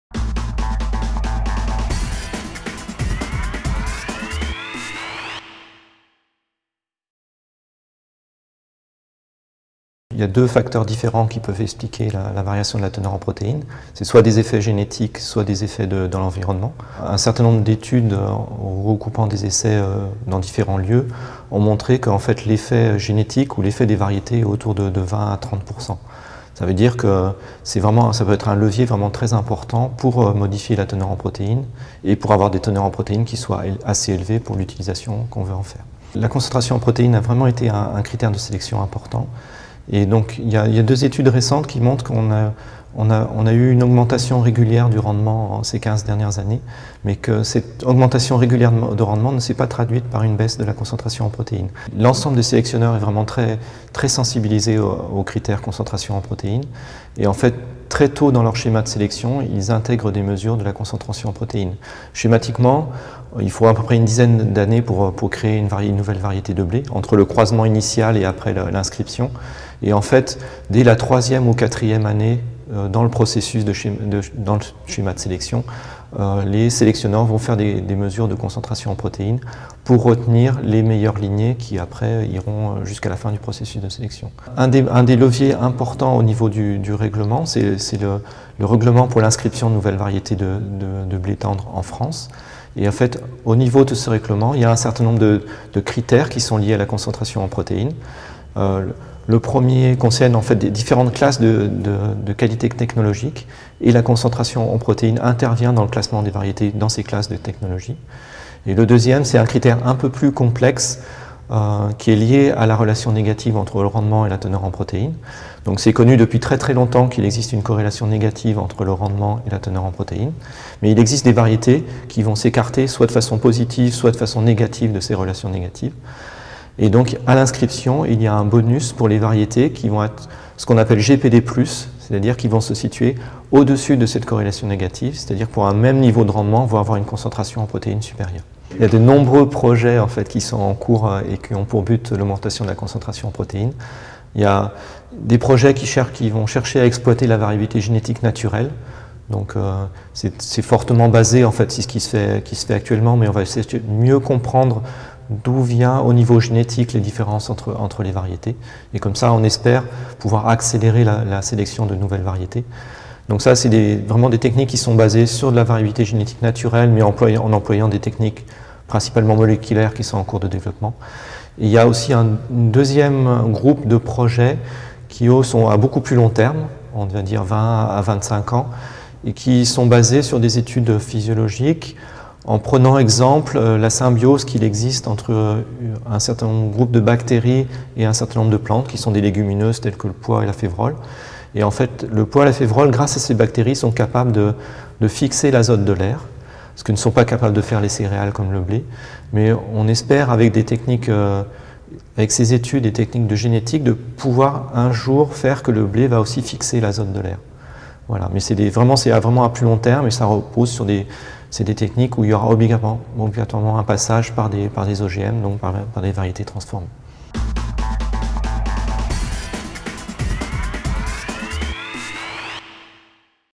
La génétique au service de la protéine du blé - ARVALIS-infos.fr
Jacques Le Gouis, Directeur de recherche à l'INRA de Clermont Ferrand, nous explique en quoi la génétique peut constituer un levier très important pour modifier la teneur en protéine du blé.

Retrouvez toute l'info technique des grandes cultures et des fourrages en vous abonnant à la lettre ARVALIS-infos.fr